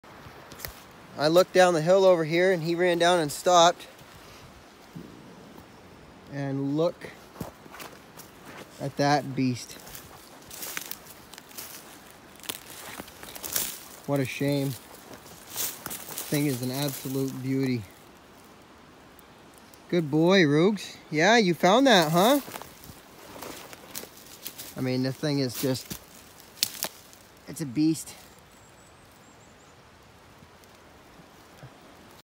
My dog found a big bull moose dead!
My Labrador retriever shed dog found a big bull moose dead while out looking for shed antlers in the spring.